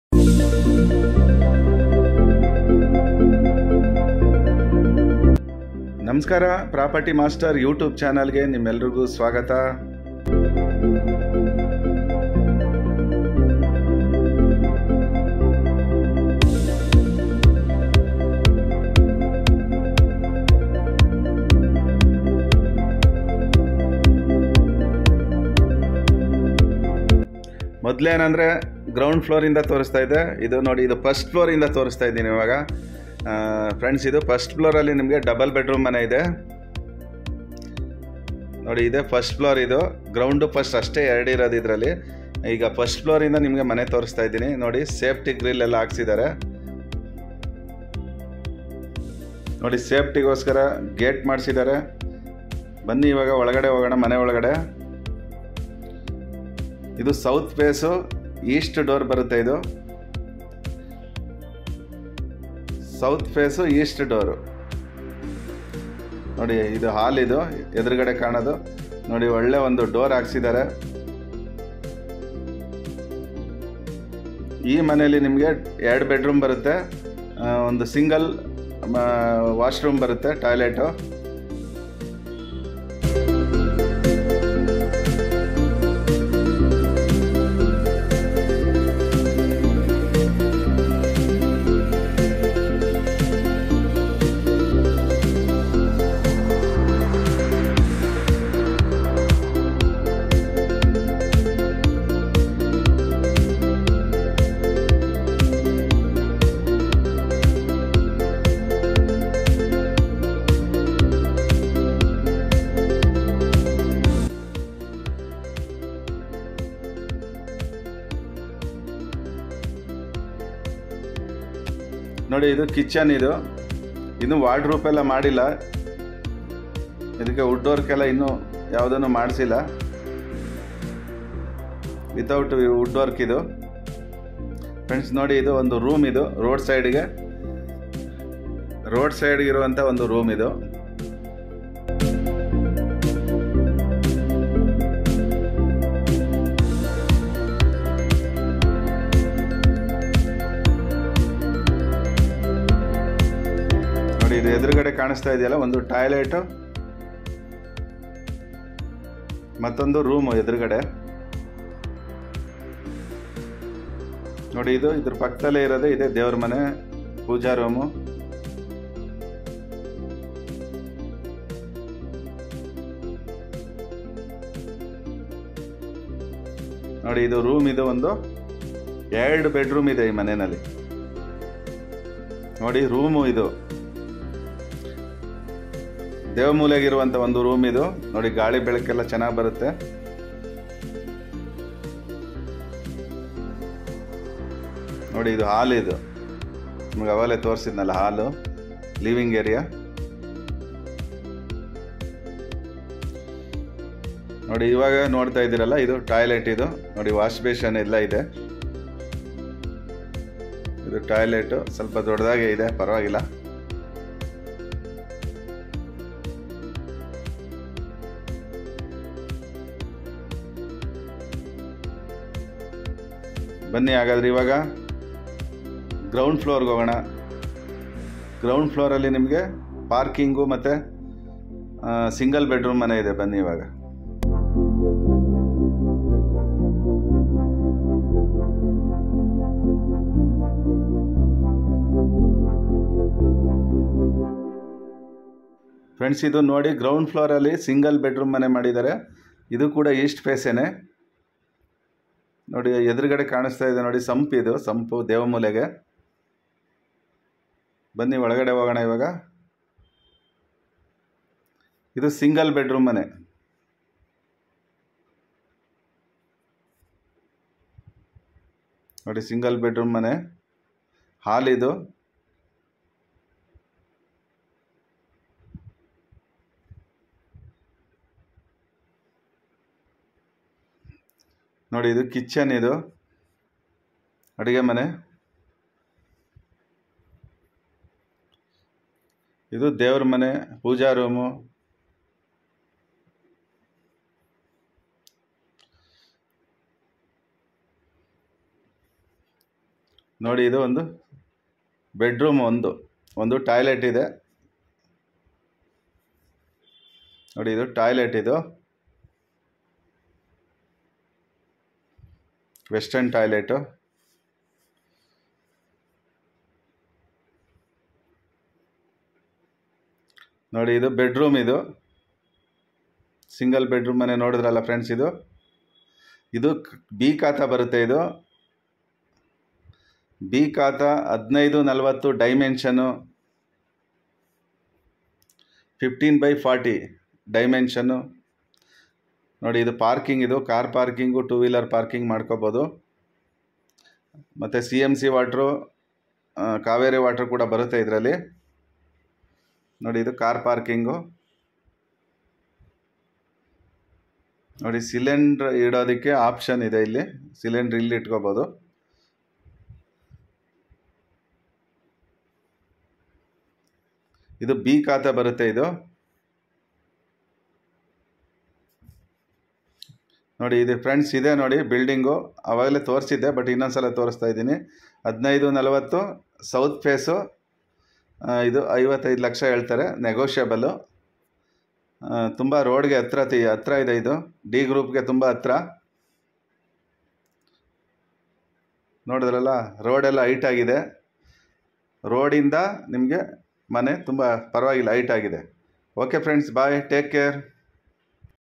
55 lakh house for sale Magadi Road property master Bangalore 55 ಲಕ್ಷಕ್ಕೆ ಮನೆ ಮಾರಾಟ 7892278544
55 lakh house for sale dimension 15 by 40
B khata South face East door

ground floor 1st floor cauvery water TMC water available
Magadi Road Bangalore near d group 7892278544

3.0

3.0